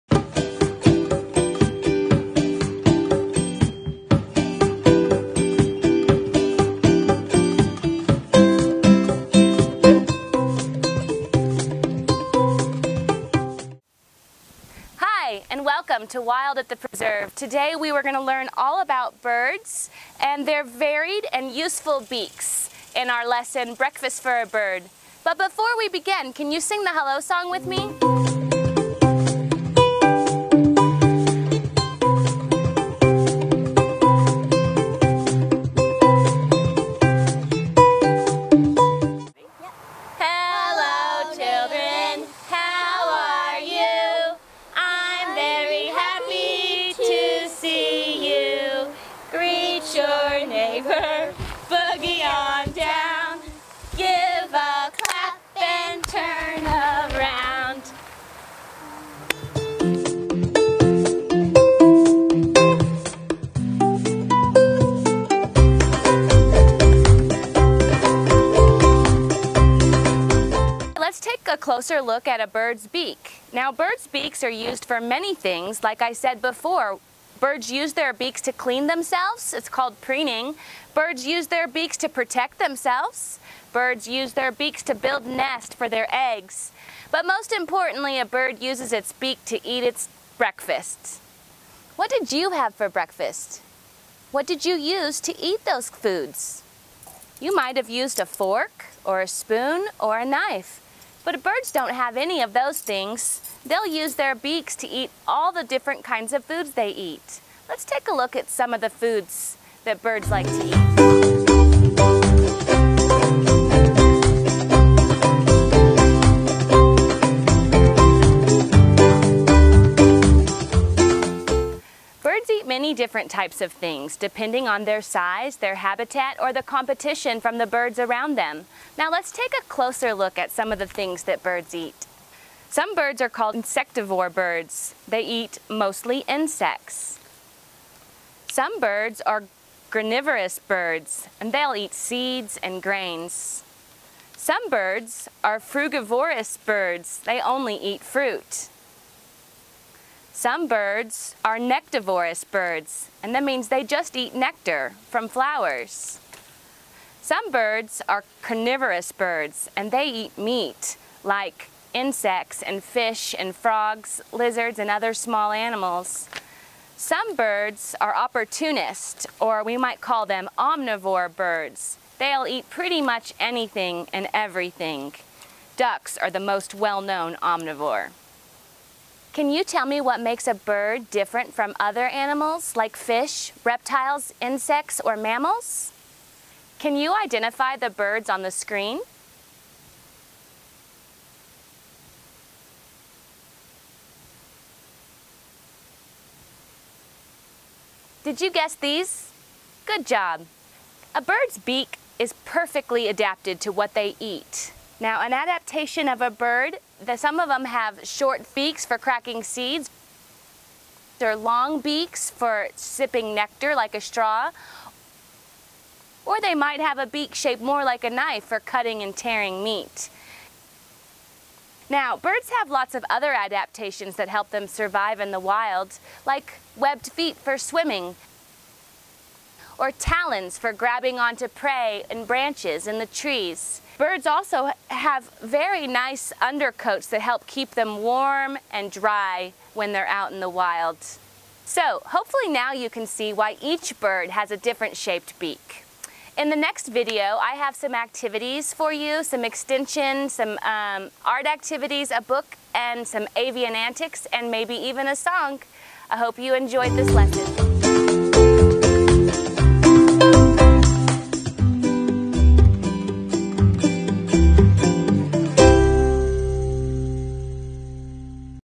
Bird Breakfast
In this video, Kari teaches us about birds and why their beaks are different shapes and sizes and what they like to have for breakfast!